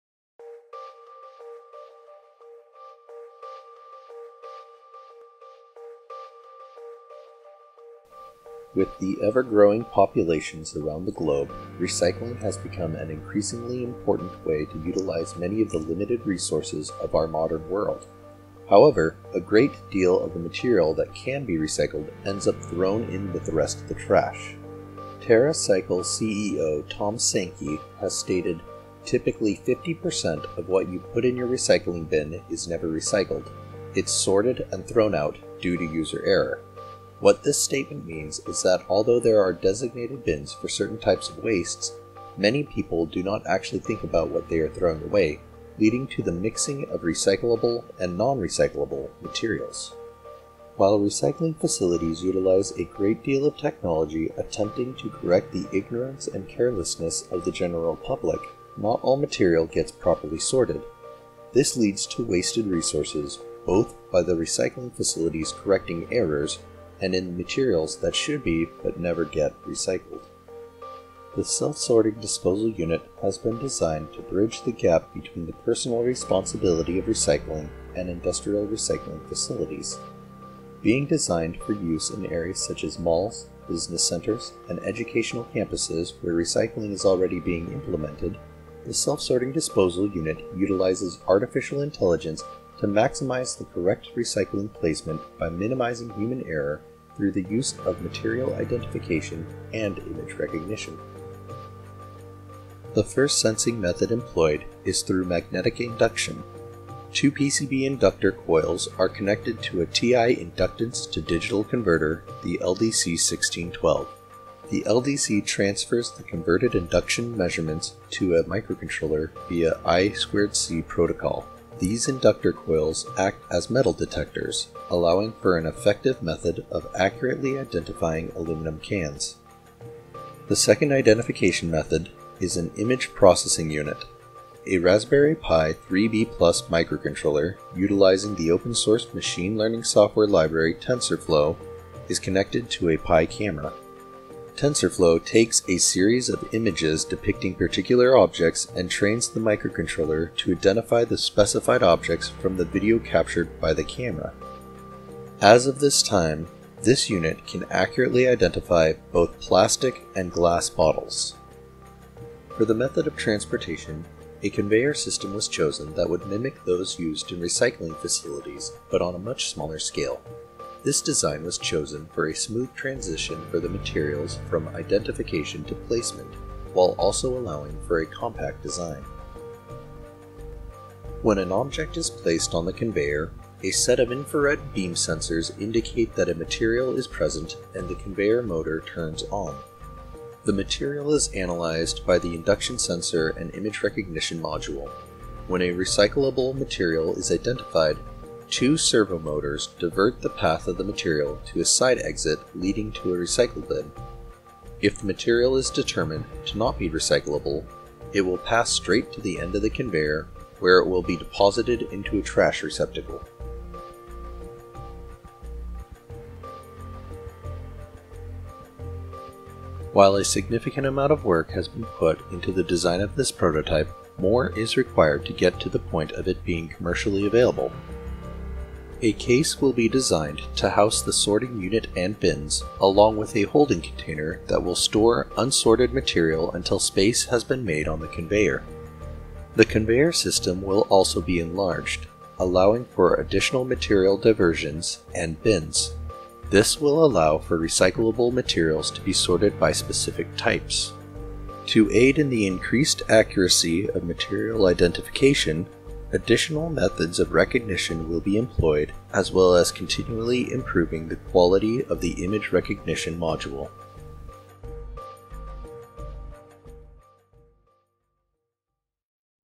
Self-Sorting Disposal Unit
The Self-Sorting Disposal Unit is a waste product receptacle designed to minimize human error in the proper disposal of recyclable and non-recyclable materials. The product is aimed for public areas such as malls and college campuses where recycling is implemented but difficult to enforce. The unit consists of storage bins for designated material types, and a automated  sorting system. The sorting system contains a infrared light to detect new items; a short term holding area for high volume, which feeds into a conveyor transport operated by a DC motor; and two material sensors which determine item placement. Because people are often distracted or preoccupied the design of the Self-Sorting Disposal Unit makes it ideal for environmentally  conscious public areas. With a single entry method, a person need only drop and go to be able to do their part in helping to recycle.